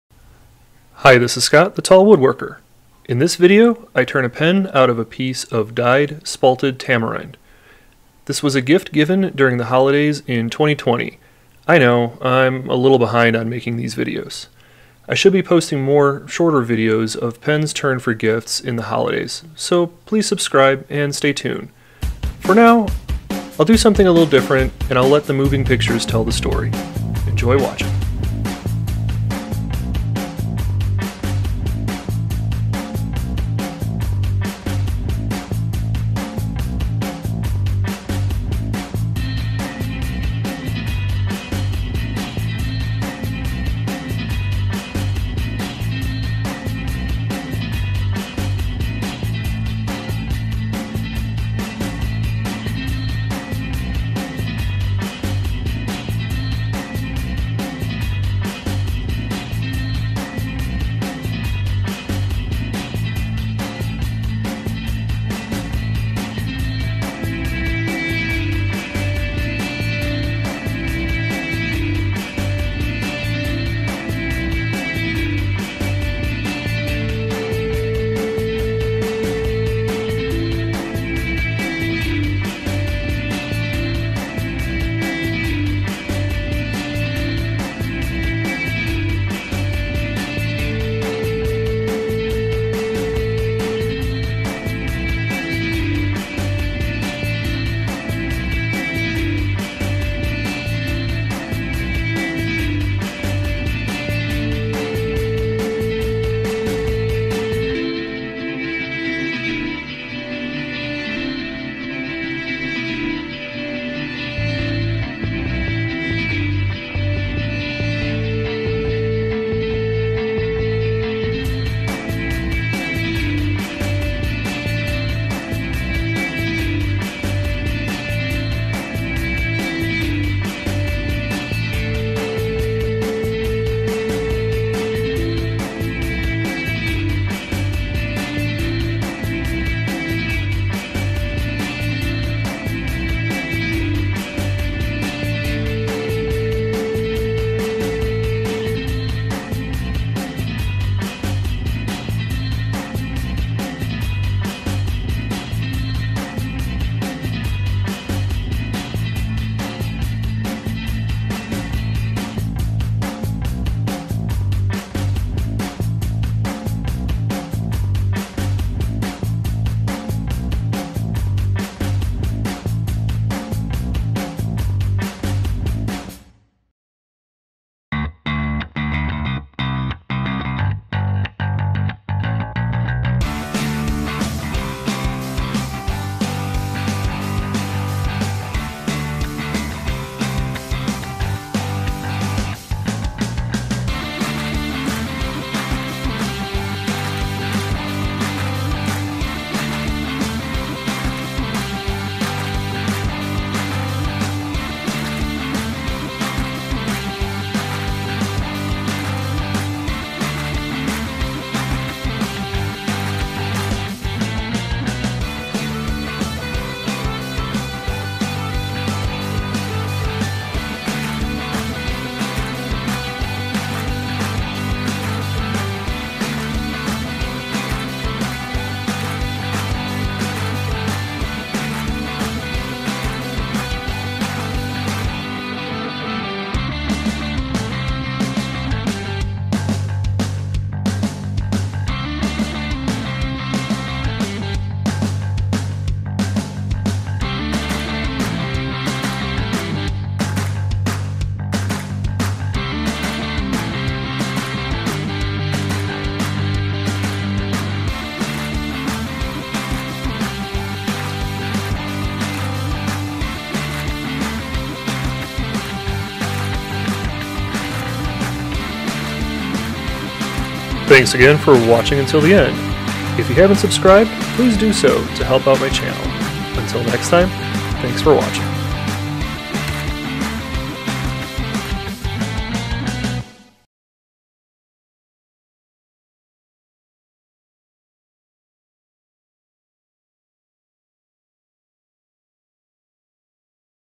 Turning a Princess Pen with Pink Dyed Spalted Tamarind
In this video, I turn a pen out of a blank of dyed spalted tamarind.  The color patterns are amazing, but my shop looked like a unicorn sneezed all over it.